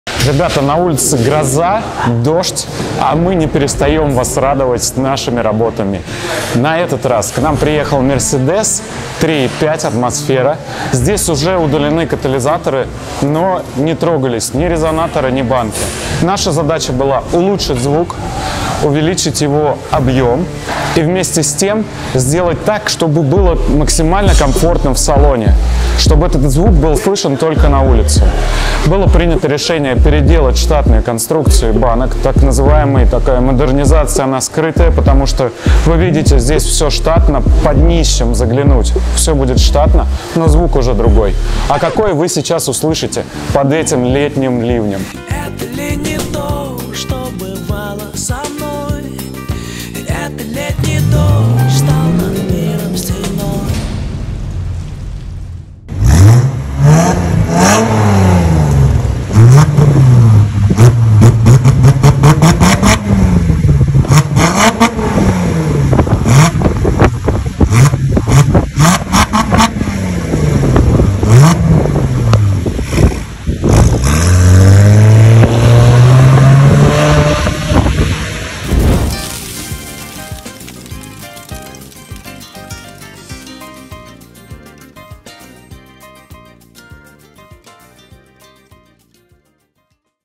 КРУТОЙ ЗВУК под AMG на MERCEDES E350🔥
От этого автомобиля трудно оторвать взгляд, но ещё труднее отказаться от соблазна прокатиться в нём👀 Заострённый дизайн его интерьера и экстерьера - это не просто дань высокому вкусу, а, скорее, старая добрая интерпретация идеи современного автомобиля, способная подчеркнуть в его облике мощь и динамичность💪 Тачка отличается молодёжностью и свежестью, по сей день актуальна, расставляет спортивные акценты посредством элегантных деталей🤌 Заняв место в этом автомобиле, вы сразу же понимаете, что оказались в совершенно особом месте, где вы наслаждаетесь не только комфортом и уютом, но ещё и сочным породистым звуком выхлопной системы🔉

✅ Приехав к нам в ultravihlop мы прокачали штатную выхлопную систему и переделали внутреннюю конструкцию банок глушителя, отрегулировав громкость выхлопа в процентном соотношении👌